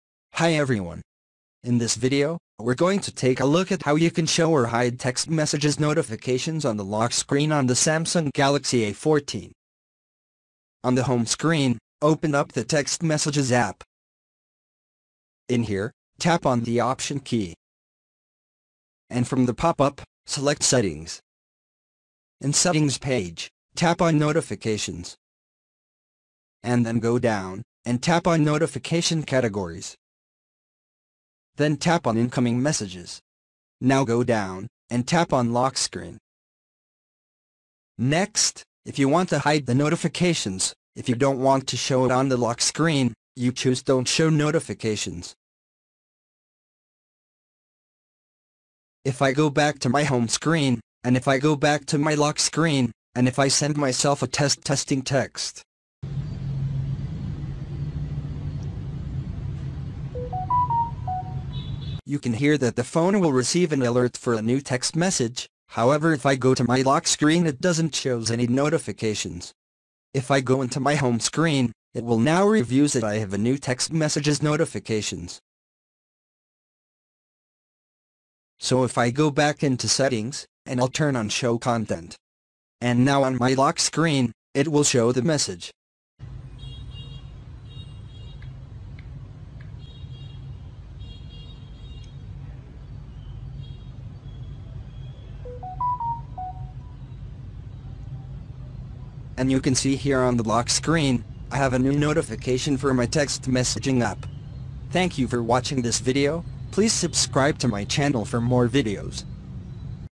Samsung Galaxy A14: How to Show/Hide Text Messages Notifications on the Lock Screen
Learn how you can show or hide Text Messages notifications on the lock screen on the Samsung Galaxy A14.